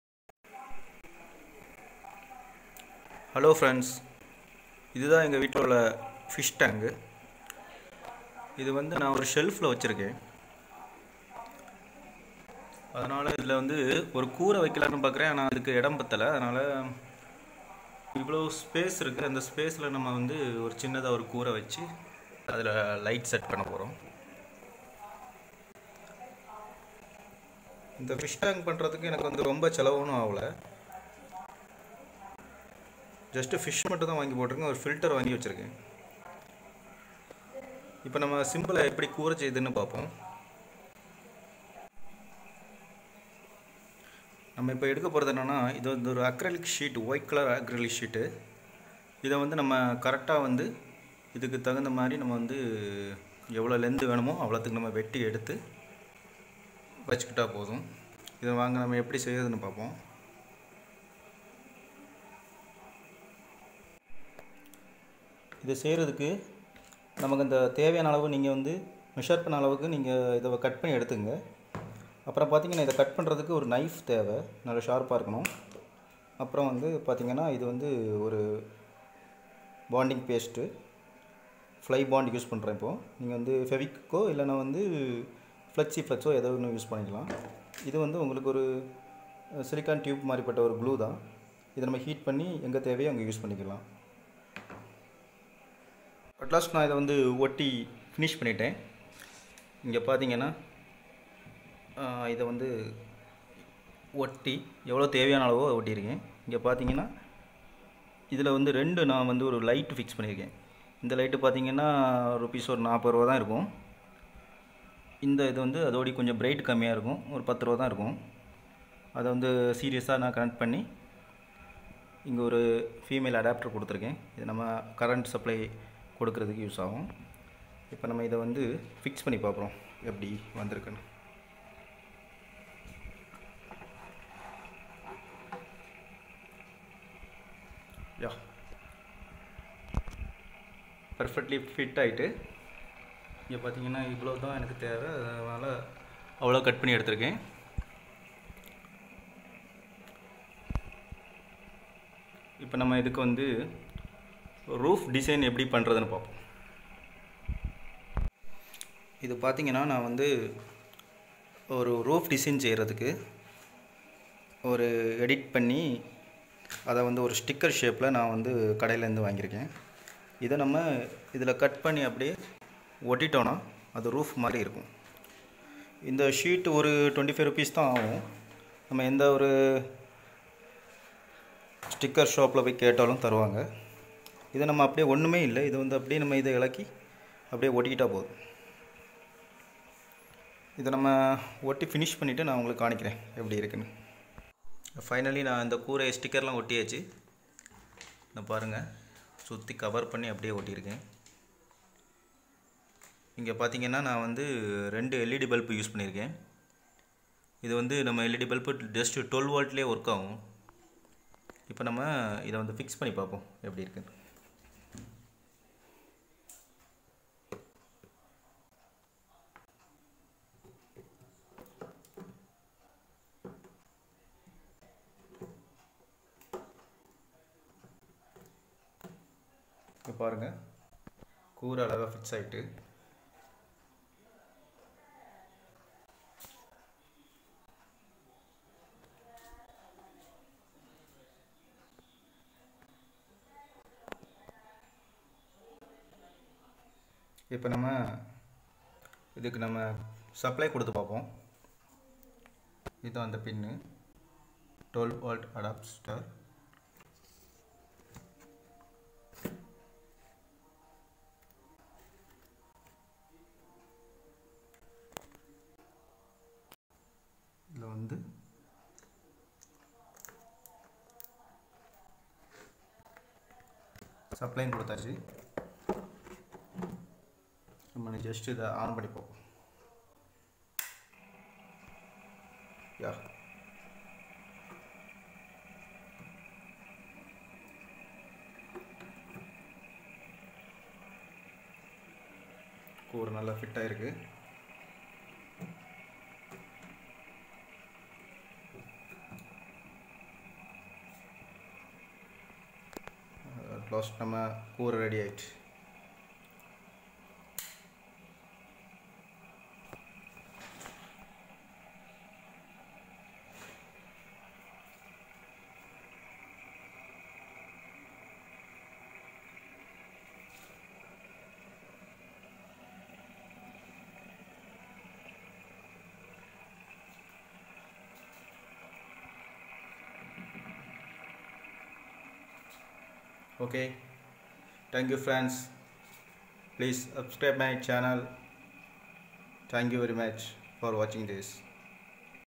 எளிய முறையில் மீன் தொட்டி கூரை அமைப்பது || Easy Way to Make Fish Aquarium Tank Roof with Light
Easy way to make Aquarium Roof with light Setup